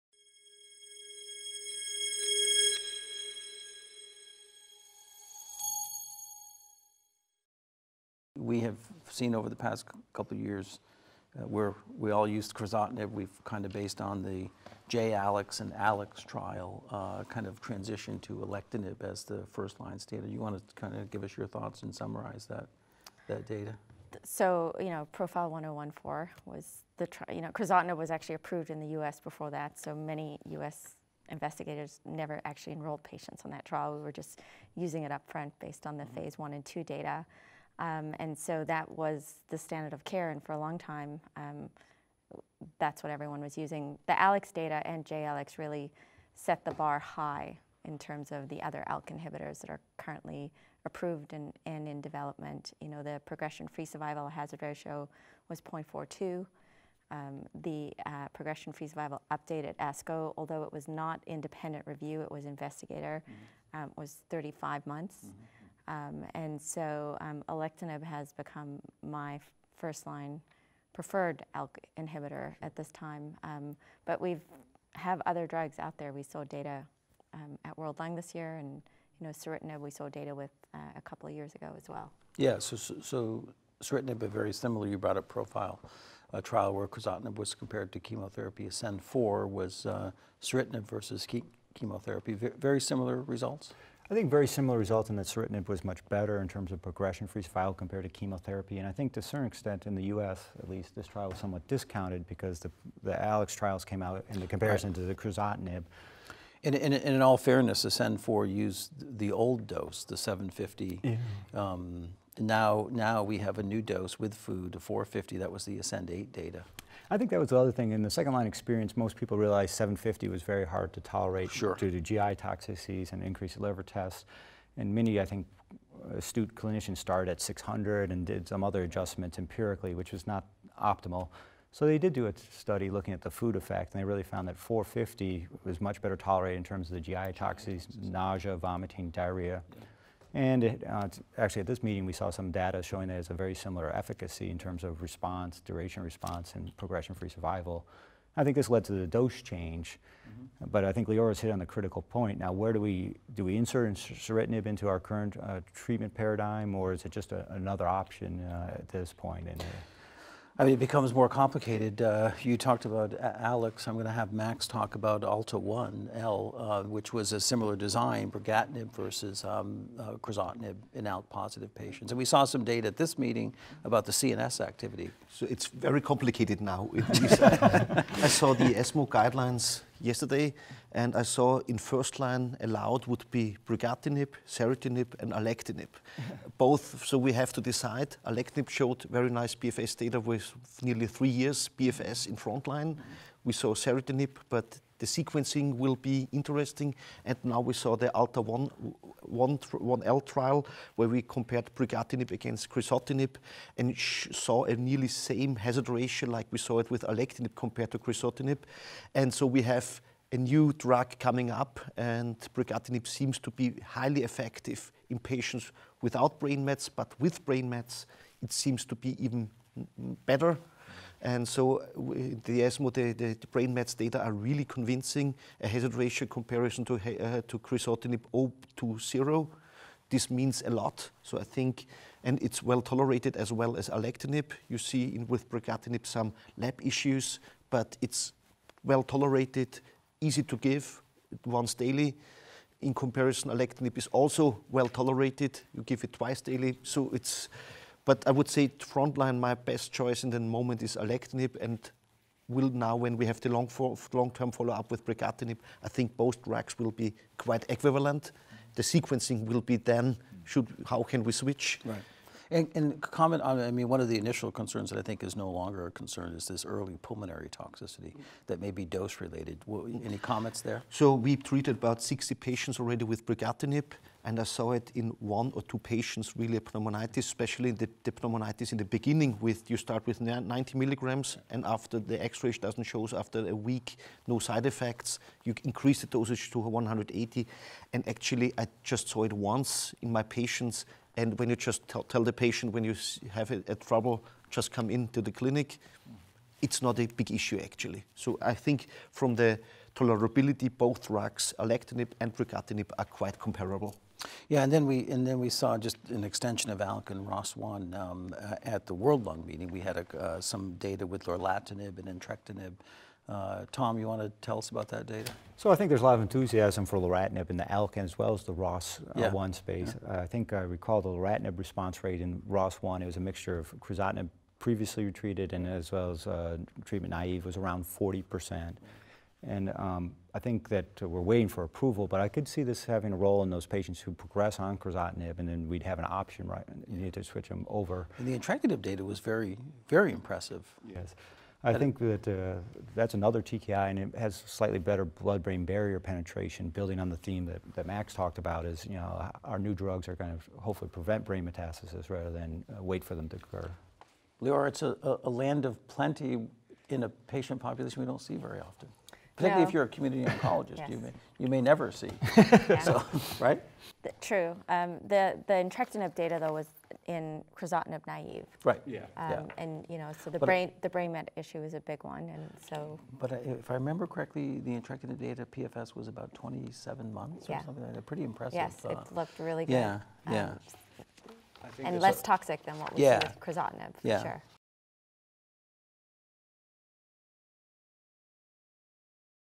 Treatment Options for ALK/ROS1 Mutation
Mark A. Socinski, MD, et al., enumerate the treatment options for patients with non–small cell lung cancer patients with an ALK /ROS1 mutation, with reference to the PROFILE 1014, ALEX, ASCEND-4/8 and ALTA-1L trials.